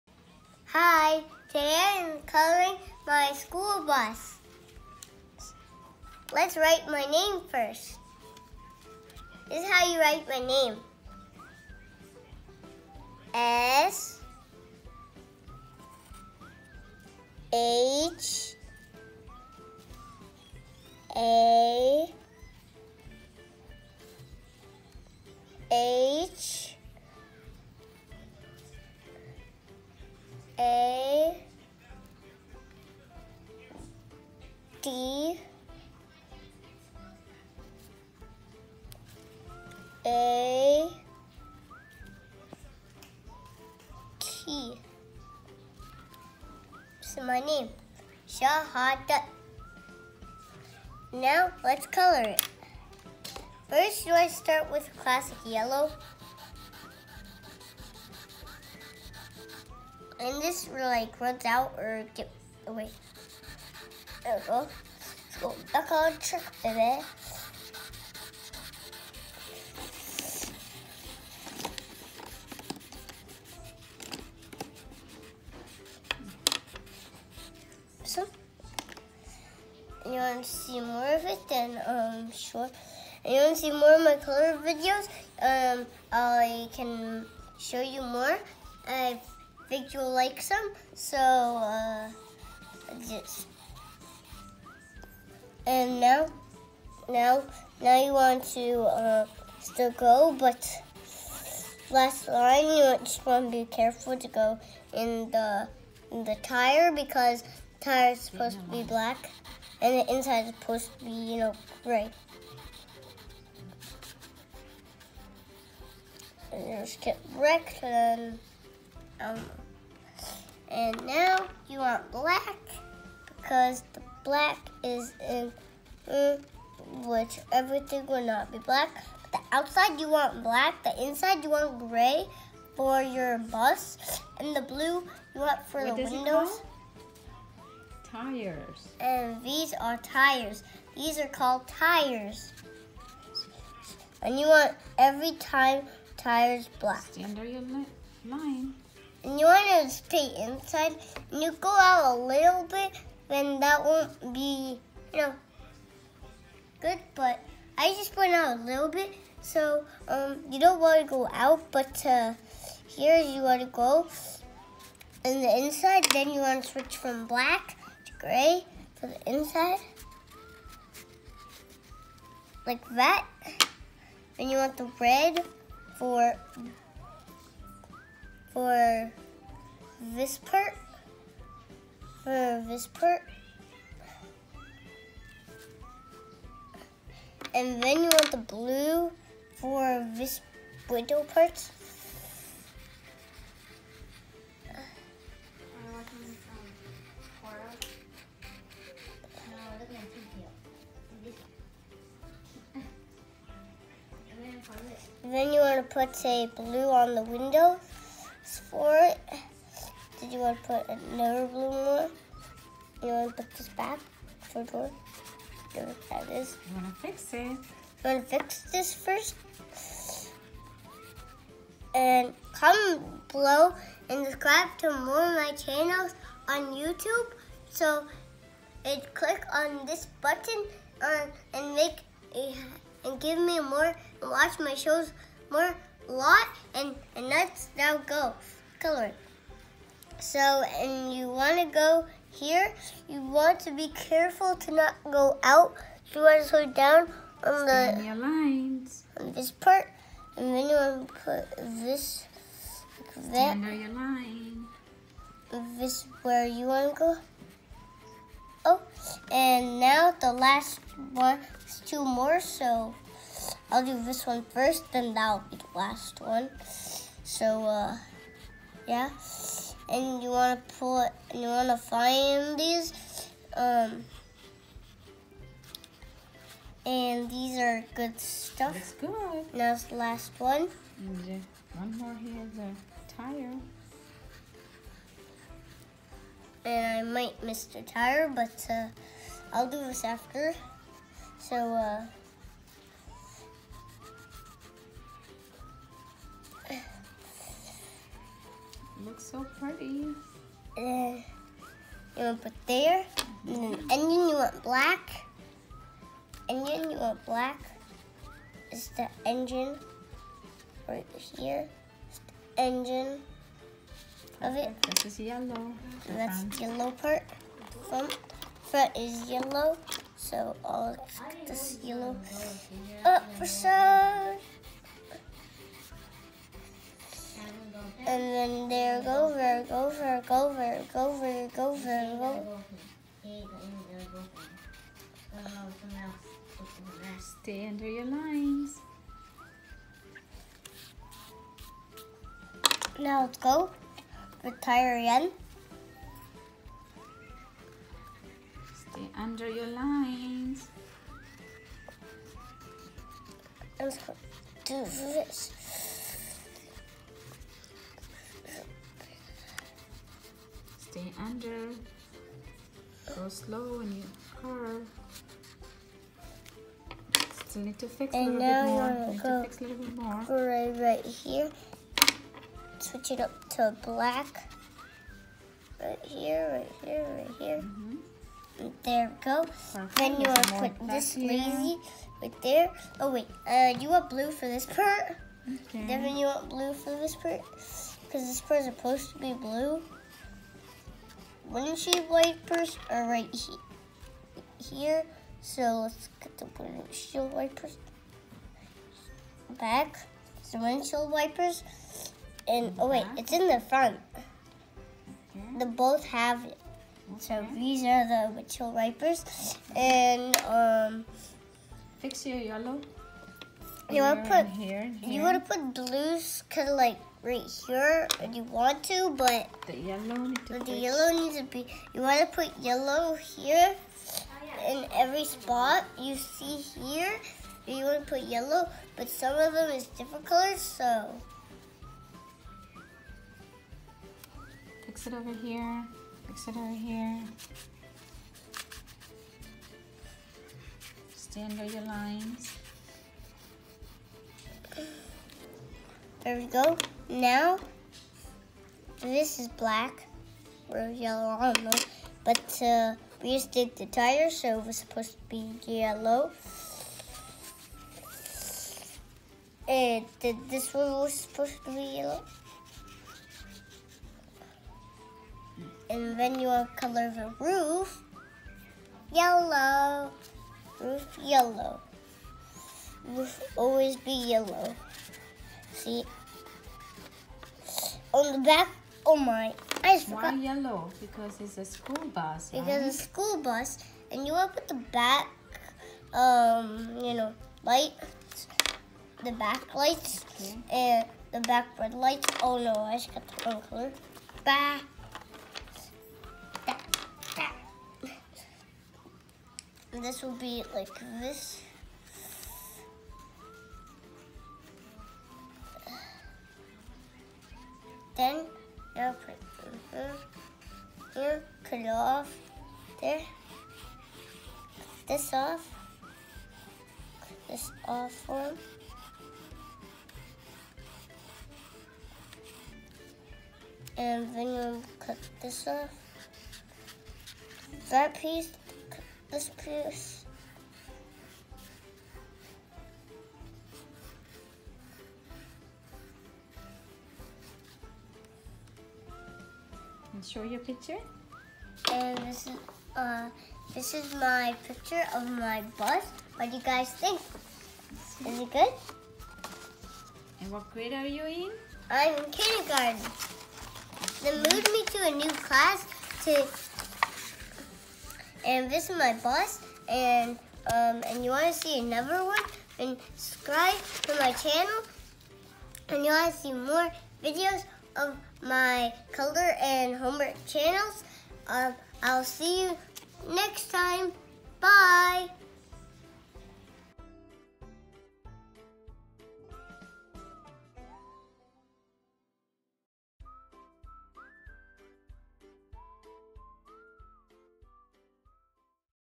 How To Color School Bus with Me| Bus coloring by Shahadat| Pre K Coloring School Bus
Kindergarten 1st quarter, see my improvement throughout the year, join in for fun